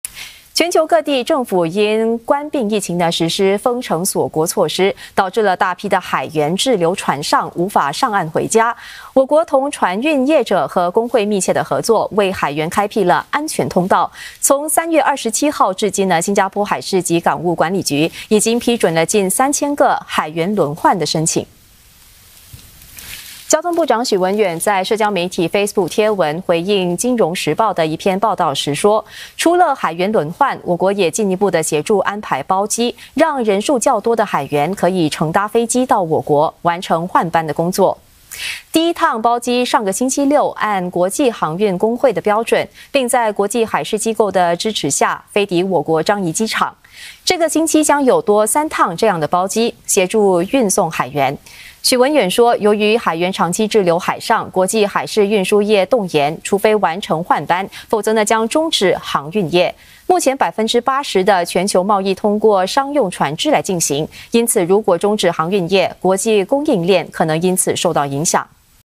大批船员因疫情滞留海上 我国已助3000船员“换班”
全球各地政府因冠病疫情实施封城锁国措施，导致大批海员滞留船上，无法上岸回家。我国同船运业者和工会密切合作，为海员开辟安全通道，从3月27日至今，新加坡海事及港务管理局已批准近3000个海员轮换的申请。